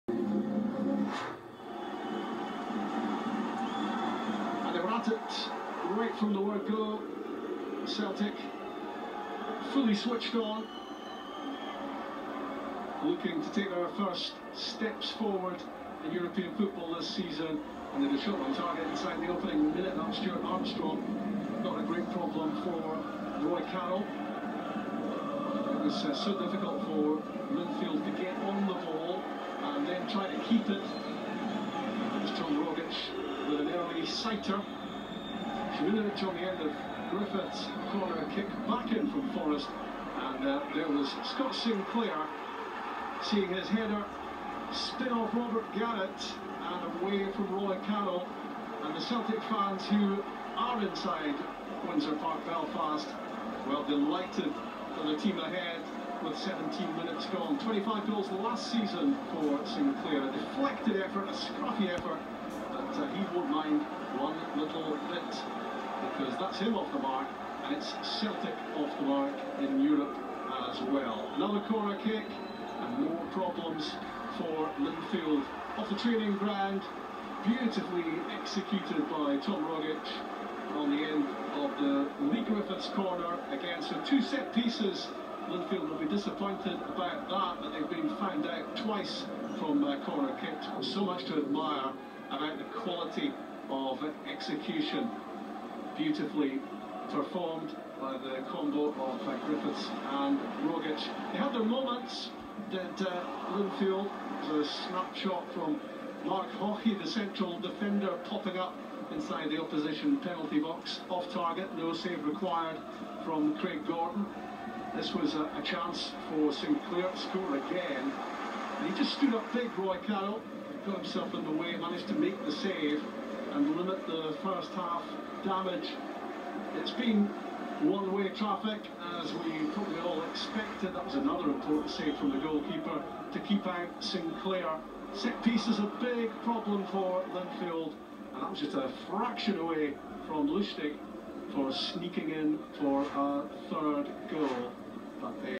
Linfeild 0-2 Celtic Champions League Qualifier 14 th July 2017 first half Highlights
Linfeild 0-2 Celtic Champions League Qualifier 14th July 2017 first half highlights

Goals Sinclair,Rogic